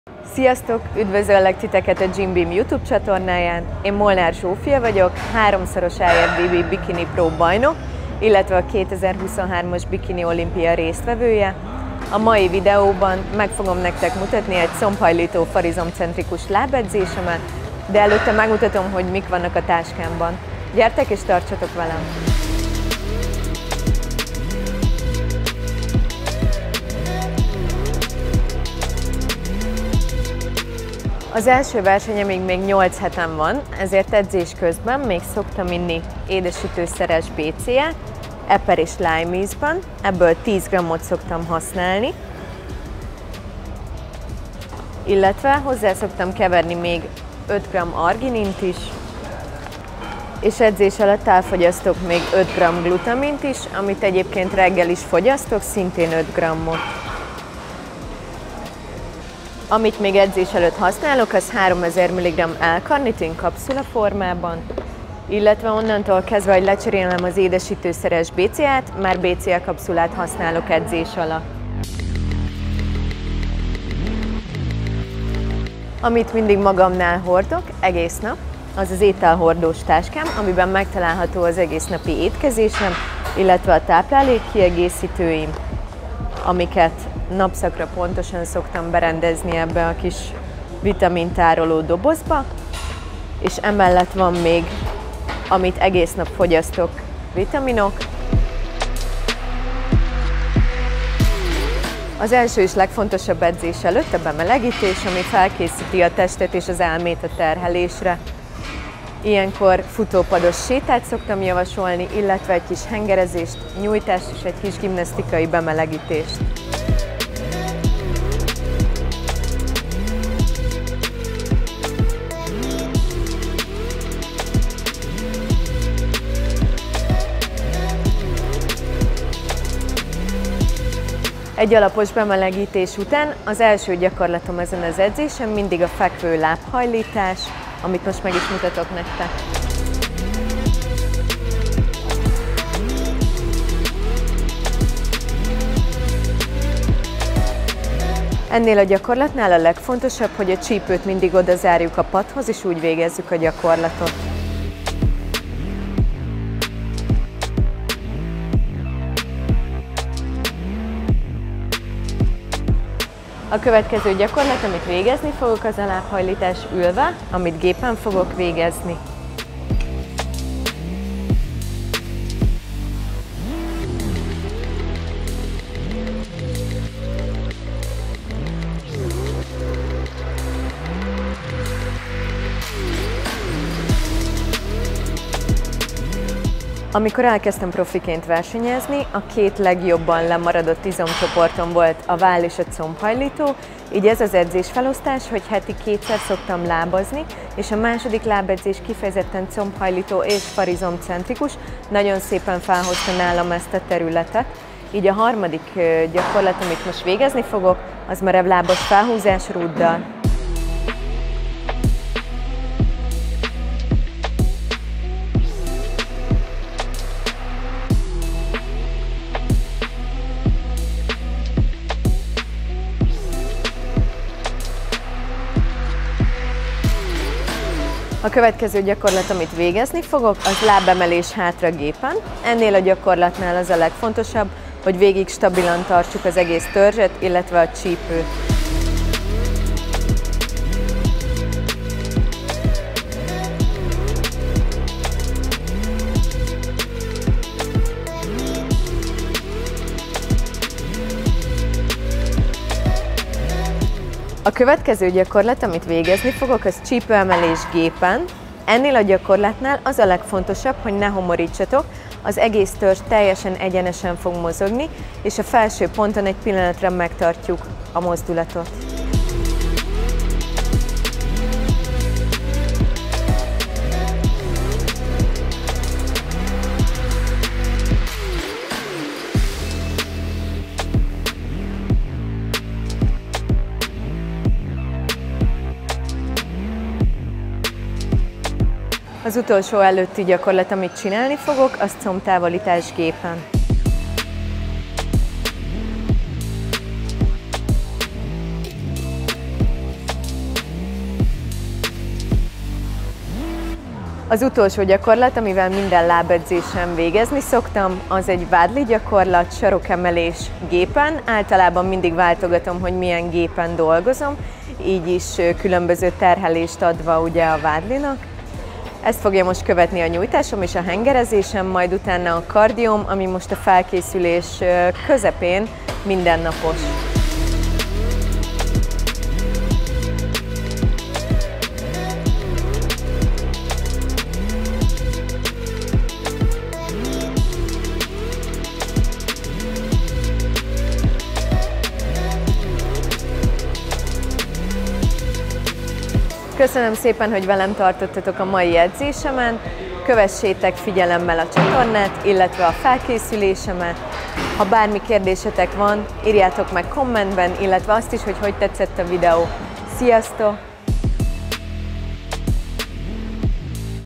Így készül Molnár Zsófi IFBB Bikini Pro a Mr Olympiára
Kemény és szépen kidolgozott popsira vágysz? 😍 Nézd meg az edzéstervünket Molnár Zsófival és tanuld meg megfelelően edzeni a popsidat. 🍑 A videóban a Mr.Olympiára készülő Zsófitól mutatunk gyakorlatokat.

Neked melyek a kedvenc popsigyakorlataid? Áruld el nekünk kommentben. 👇 Ha tetszett a videó, támogasd egy like-kal és egy megosztással.

GymBeam - Eshop progresszív élelmiszerekkel